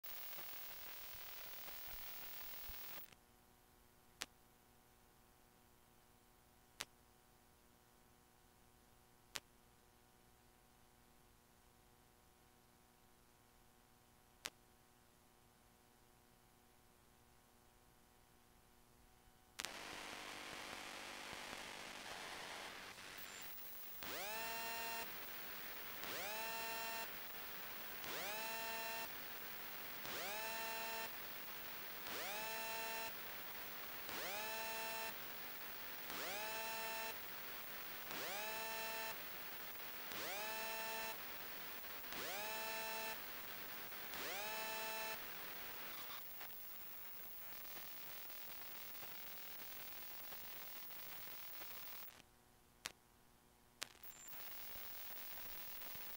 Phone ringing Sound Effects - Royalty Free Sounds
Phone ringing: A crisp and clear sound of a phone ringing, perfect for use in various multimedia projects.

Remember, all our sounds are free. If you use them, a "thanks" in the comments would be appreciated!